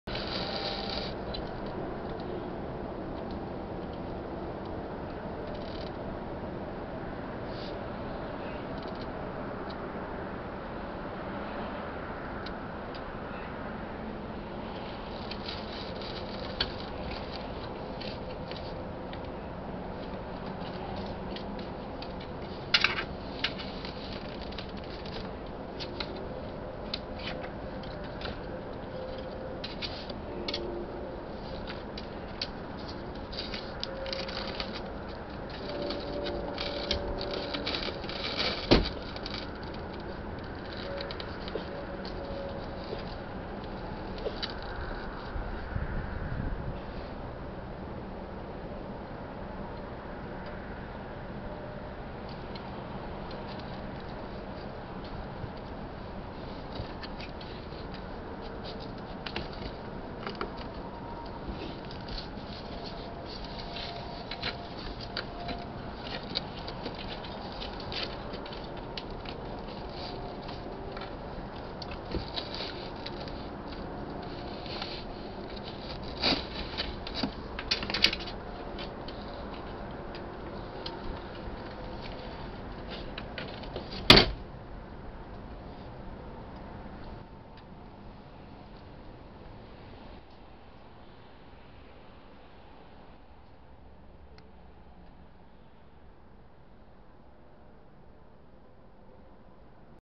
Raccoon Leaving Chimney
Greenshield Pest Control got a trail cam! Here's our first video of a raccoon exiting a chimney. What a great climber this mamma is.